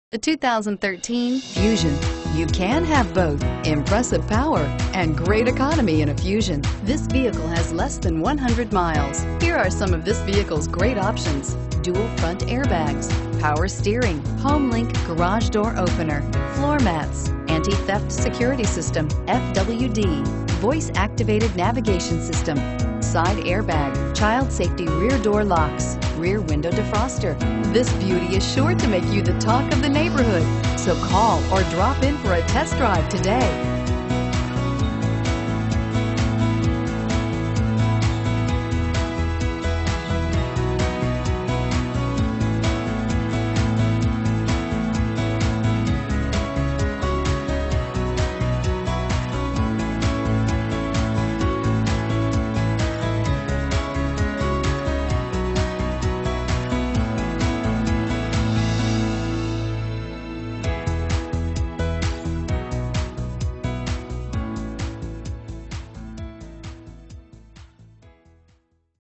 2013 Ford Fusion Greeley CO R67004
Don't miss this 2013 Ford Fusion. It's equipped with Automatic transmission and features a White Platinum Metallic Tri-Coat exterior. With 15 miles, You'll want to take this car home. Make a great choice today.